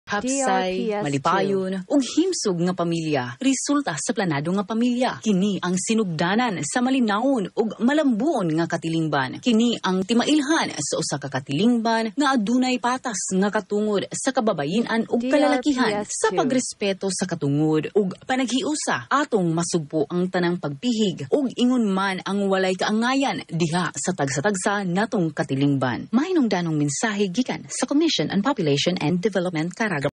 Commission on Population and Development CARAGA Region Radio PSA 2022 30s
This is the Regional Radio Public Service Announcement for Commission on Population and Development CARAGA Region for 2022.

DRPS Two 2391: Commission on Population and Development CARAGA Region Radio PSA 2022 30s